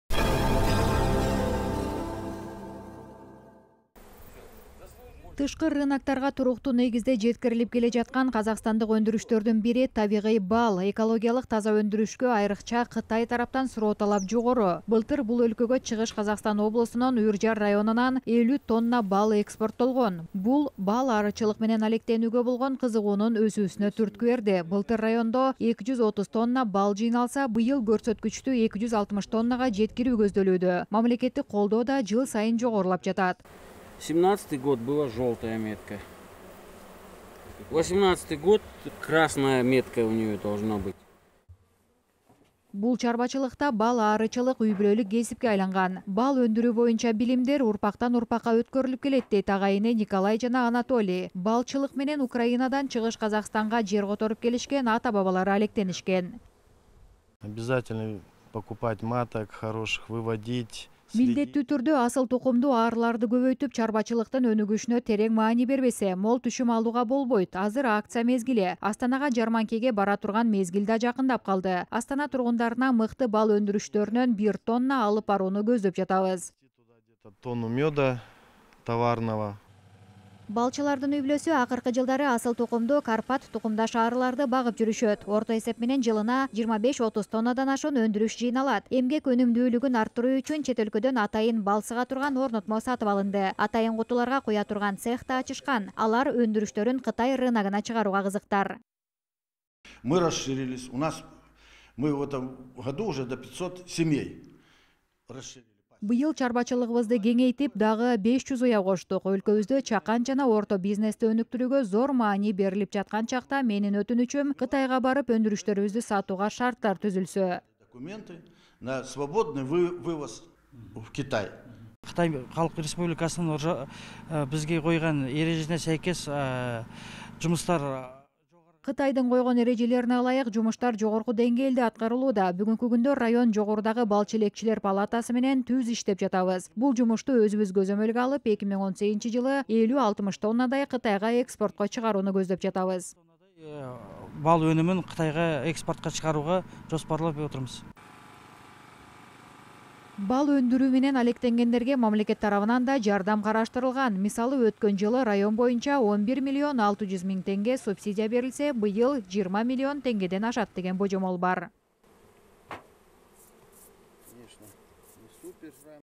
Бал аарычылык өнүгүүдө
Экологиялык таза өндүрүшкө айрыкча Кытай тараптан суроо-талап жогору.